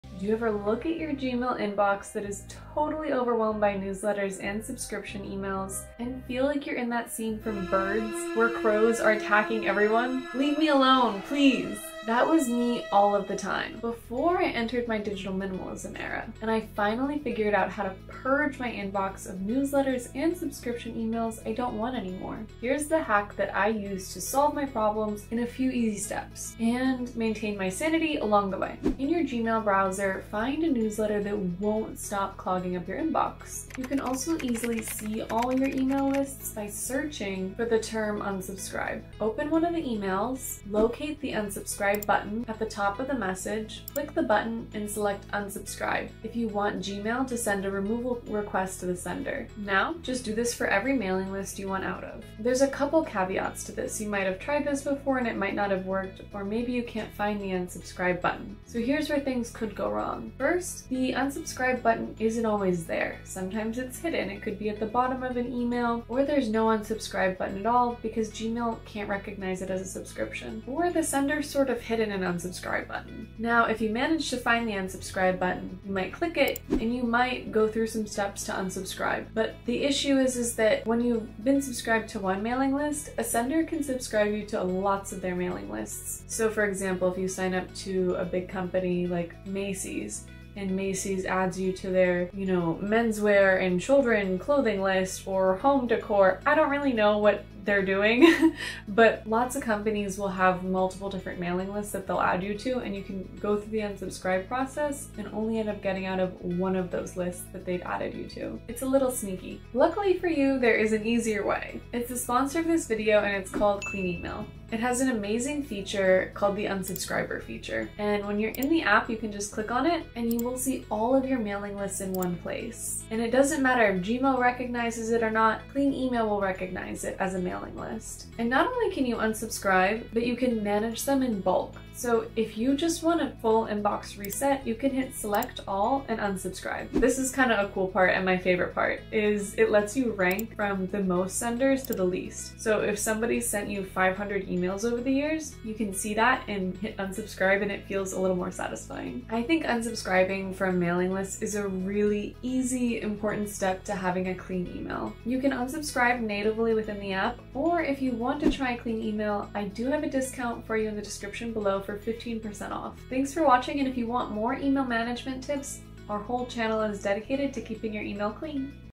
How to Mass Unsubscribe in Gmail (Without Losing Your Mind)
In this video, I’ll show you exactly how to mass unsubscribe in Gmail without losing your mind. Overwhelmed with newsletters? Wondering is there a way to mass unsubscribe from emails in Gmail? I’ll walk you through Gmail’s built-in unsubscribe option, explain how to mass unsubscribe from emails in Gmail, and share a simple tool I use for one-click unsubscribing.
Timestamps:
00:00 – Intro
00:12 – Why inbox clutter feels overwhelming
00:31 – Gmail’s built-in unsubscribe option
00:53 – Limitations of Gmail unsubscribe
01:53 – How to do a mass unsubscribe in Gmail the easy way
02:40 – Final thoughts + discount
💬 Was this video helpful? Drop your favorite email decluttering tip in the comments!
More About This Video:
Gmail’s unsubscribe button works, but it’s slow and unreliable:
You have to open and unsubscribe from each email one at a time.
Clicking links in suspicious messages can expose you to phishing.
Some senders ignore requests or add you to other lists.
There’s no true mass unsubscribe option—filters only hide emails, they don’t stop them.
That’s why I recommend using a dedicated inbox management tool. It lets you unsubscribe from multiple newsletters in bulk, blocks senders that won’t stop, and can even delete past emails while the unsubscribe request is being processed. Plus, features like Smart Folders and a Read Later option keep your inbox organized automatically.
🔹 Take control of your inbox today!
💬 I’d love to hear in the comments if these tips worked for you.